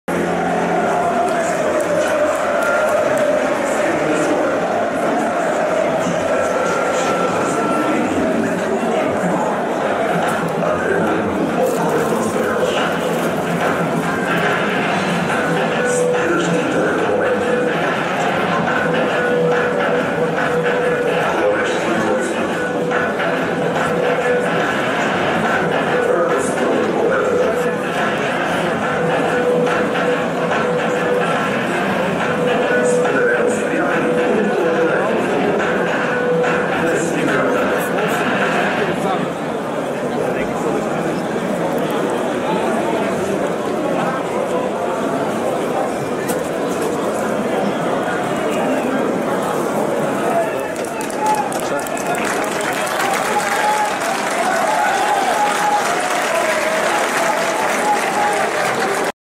RESPECT! Bulgarian Captain I.Popov talking to the fans to stop the racist abuse against England 0-6
The Bulgarian Captain Ivelin Popov is trying to stop the racist abuse on the English players!He is talking with his own fans trying to convince them to stop it!That's something we can't see everyday and has my full RESPECT!NoToRacism